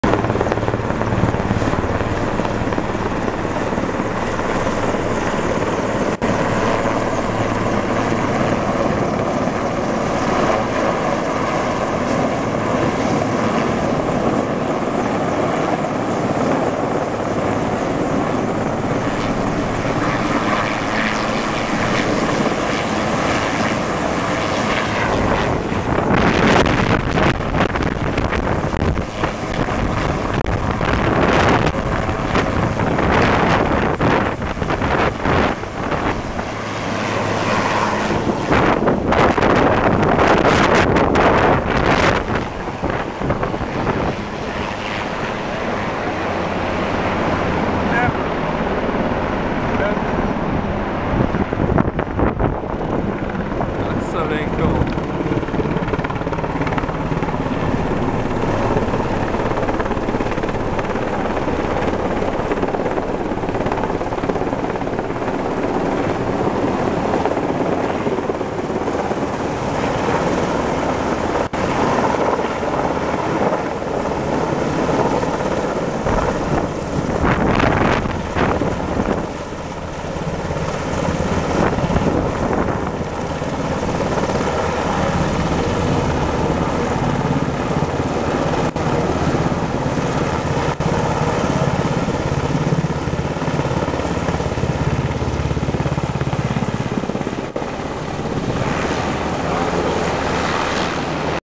Couple medium helicopters dipping out of the South Umpqua River
A ? and a Black Hawk
Stouts Creek Fire, August 2015